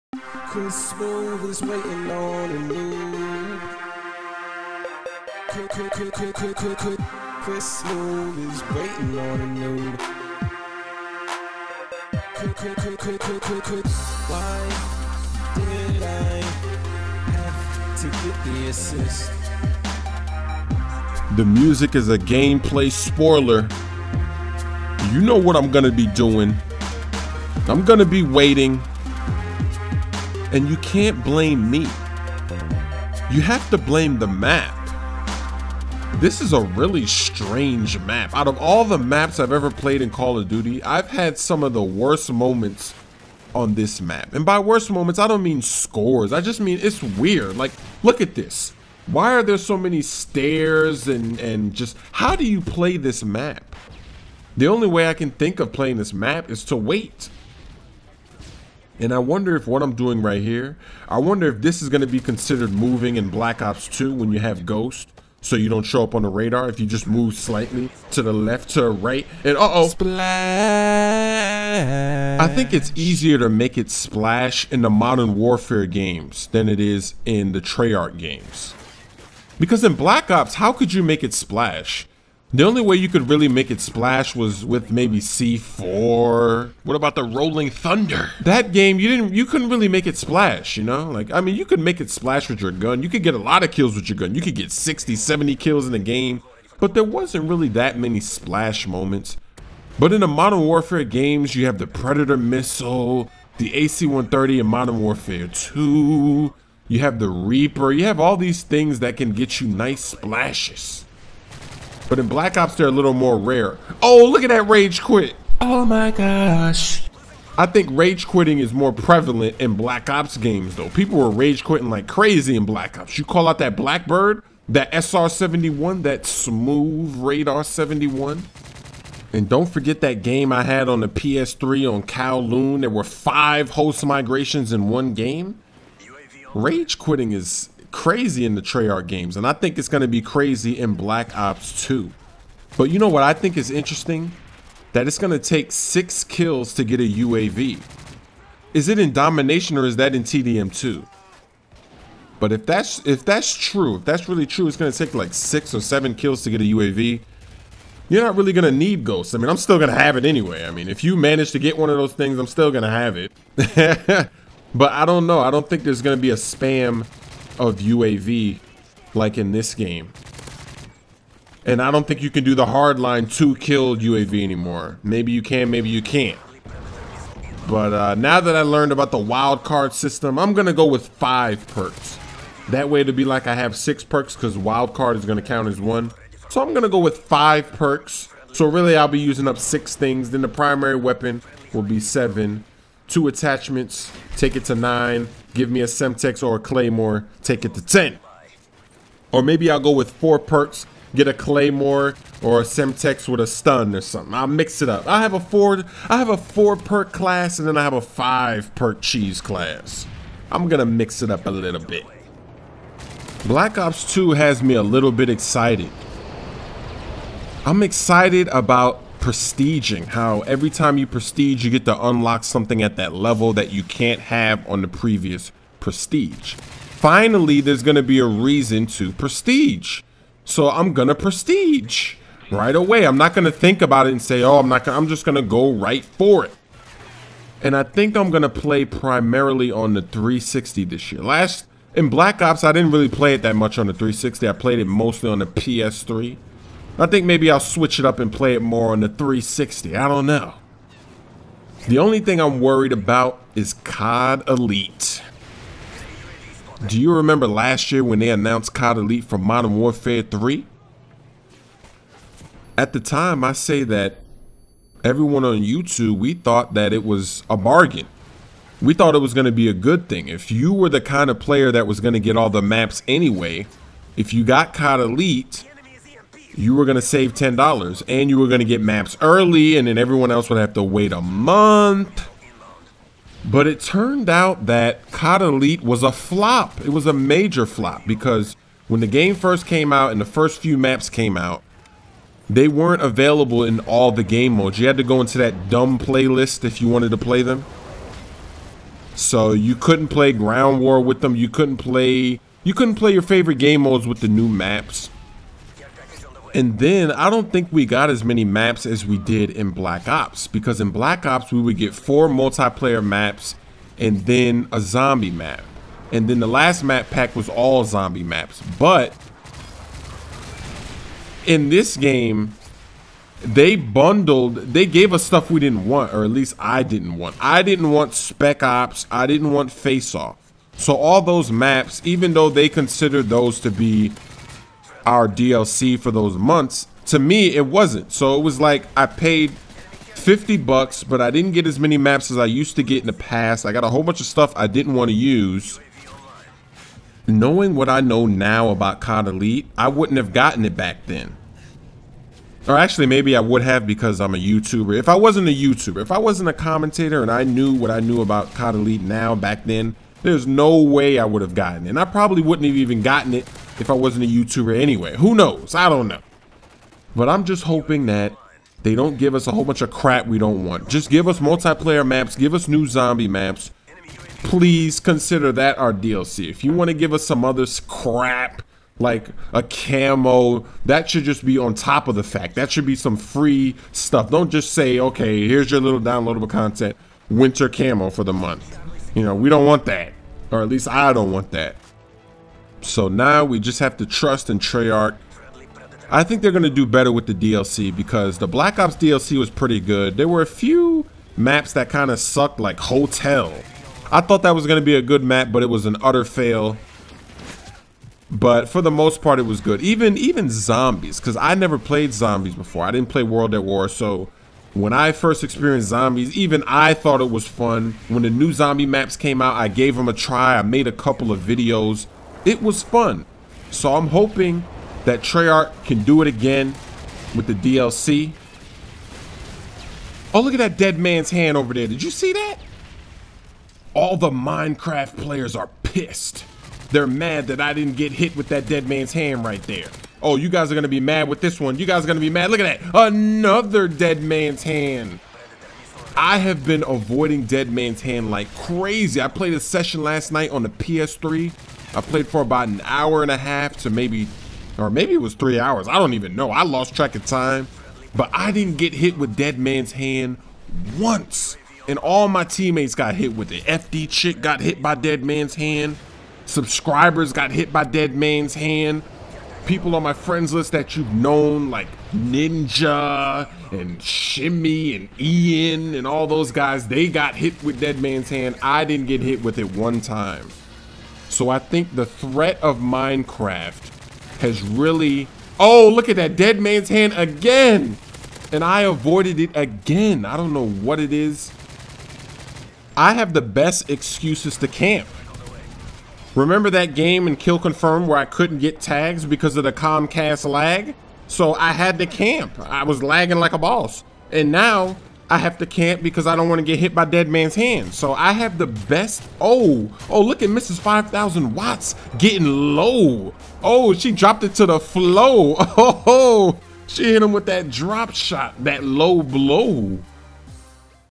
COD MW3 - COD Elite
Remember last year when we thought COD Elite would be a bargain? Will Treyarch do it right?